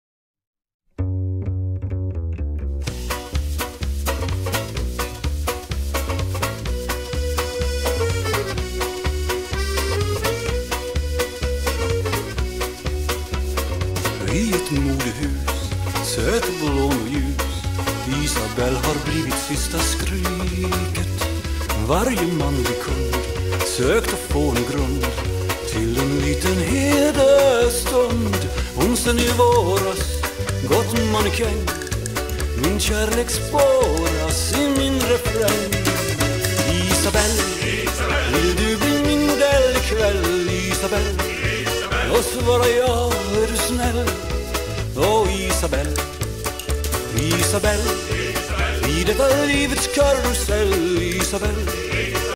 Agilityträning med Flynn 110511
Lite agilityträning hemma med unghunden Flynn. 110511